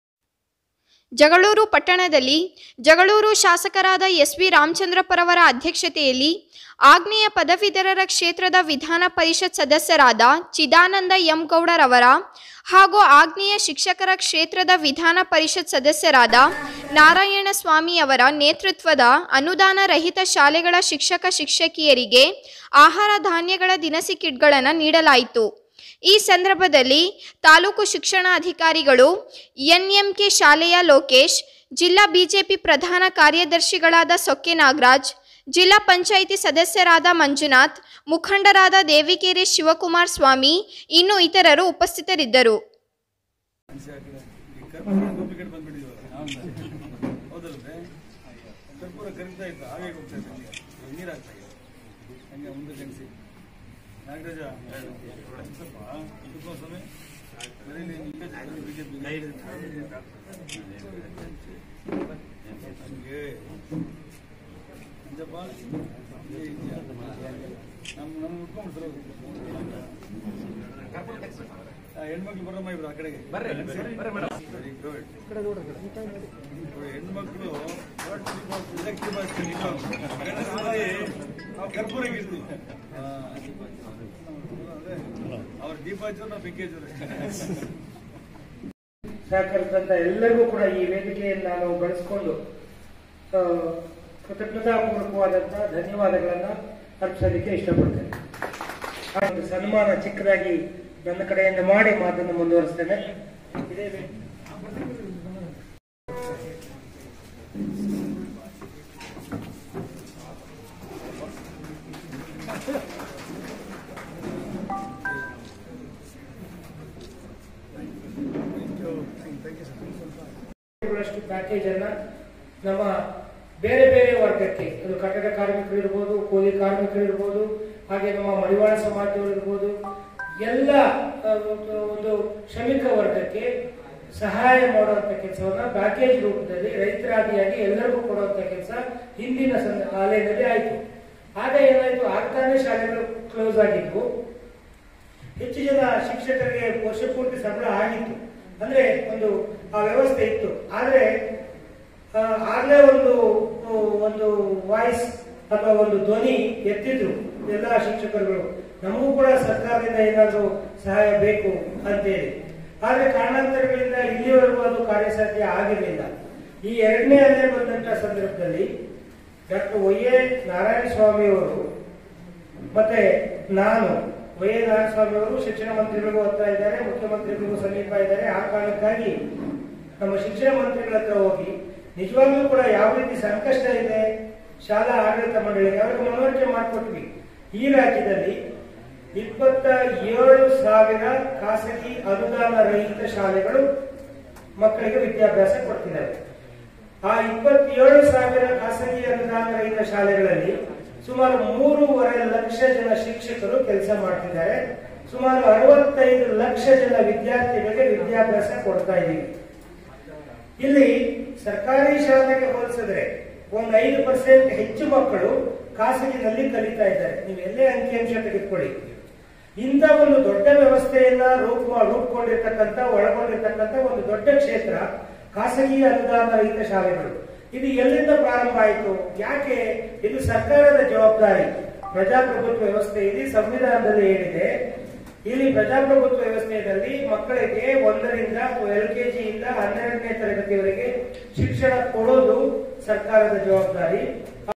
ಅನೂದಾನ ರಹಿತ ಶಾಲೆಗಳ ಶಿಕ್ಷಕ ಶಿಕ್ಷಕಿಯರಿಗೆ ಅಹಾರ ದಾನ್ಯಗಳ ದಿನಸಿ ಕಿಟ್ ಗಳನ್ನ ನೀಡಲಾಯಿತು
ಜಗಳೂರು